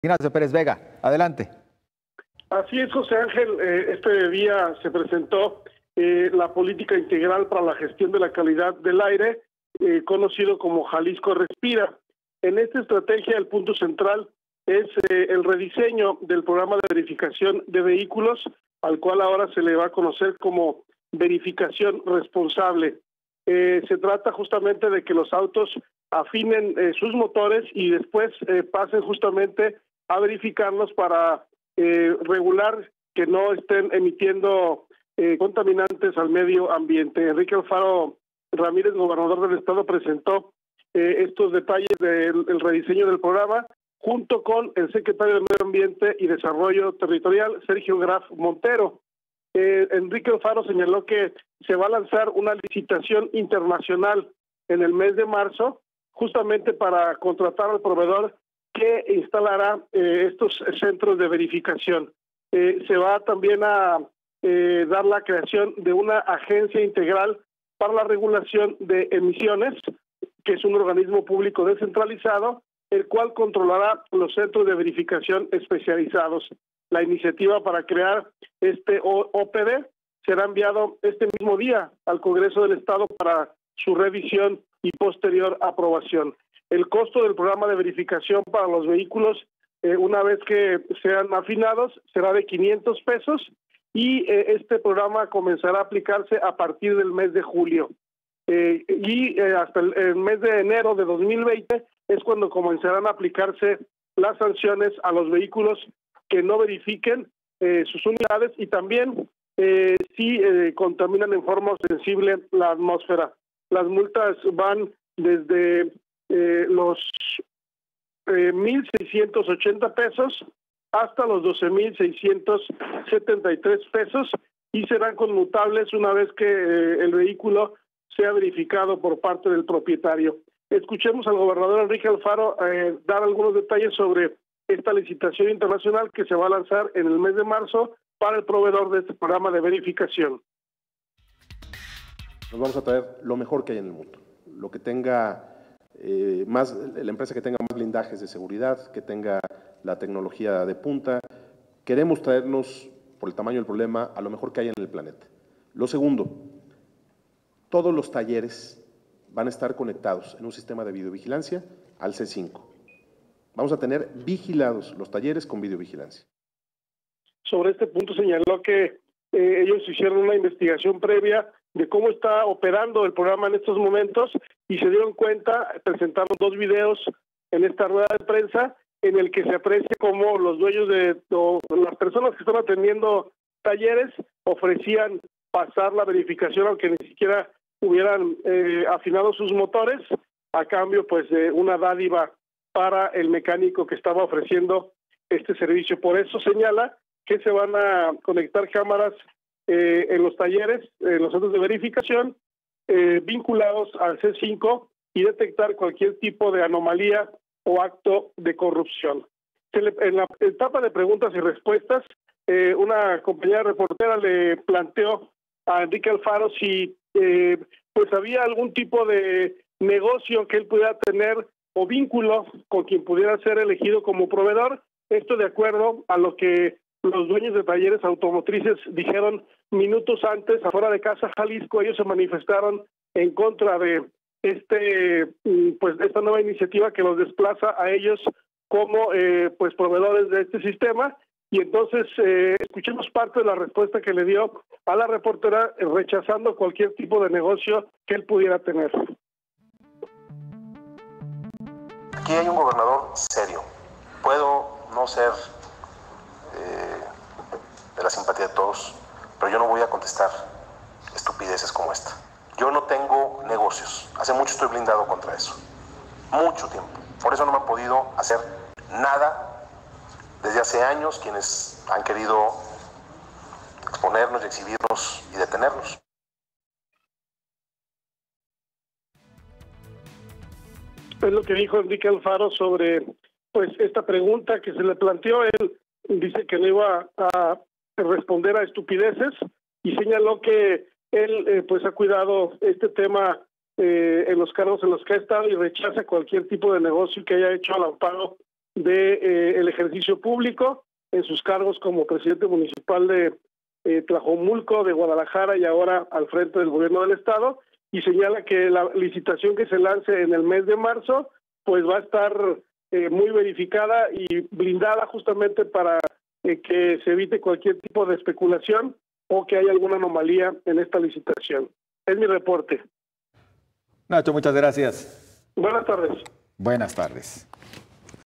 El nuevo programa de verificación vehicular iniciará hasta el verano de este año
El nuevo programa de verificación vehicular iniciará hasta el verano de este año, adelantó el gobernador Enrique Alfaro.